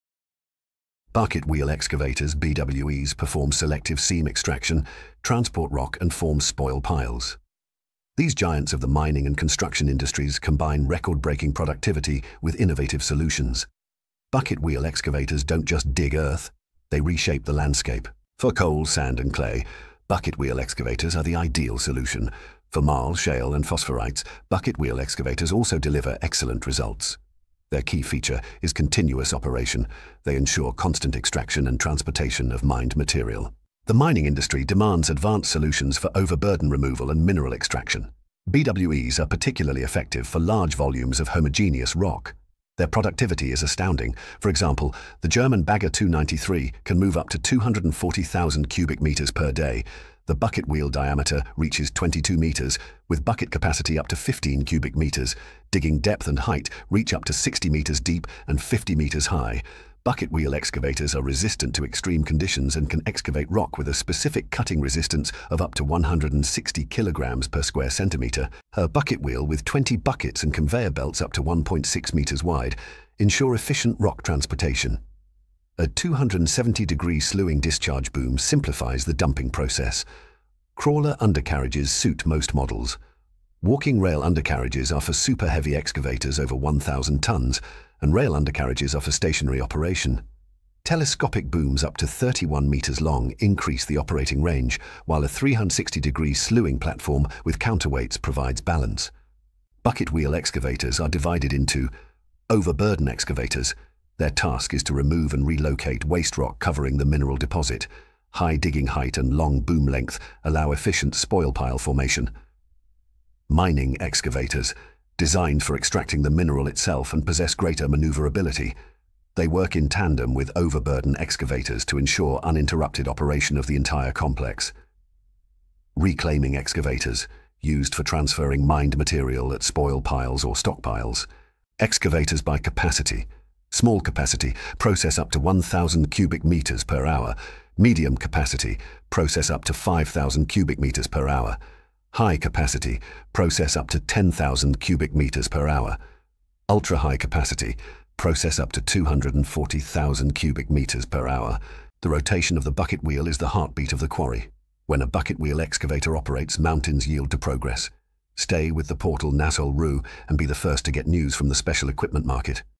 Bucket wheel excavators rewrite the landscape
Bucket wheel excavators are giants of the mining and construction industries, reshaping the landscape rather than simply digging earth. They perform selective seam extraction, transport rock, and form spoil piles. These machines combine record-breaking productivity with innovative solutions. They are ideal for coal, sand, and clay, and deliver excellent results with marl, shales, and phosphorites.
The key feature of bucket wheel excavators is their continuous operation, ensuring constant extraction and transport of mined material. Their productivity is astounding: Germany's Bagger 293 can move up to 240,000 cubic meters per day! The bucket wheel diameter reaches 22 meters, with a bucket capacity of up to 15 cubic meters.
Excavators are categorized into overburden, extraction, and transfer types, each performing its unique tasks. The rotation of the wheel is the lifeblood of the quarry. When a bucket wheel excavator operates, mountains yield to progress.
Stay with NASHYOL.RU and be the first to get news from the special equipment market!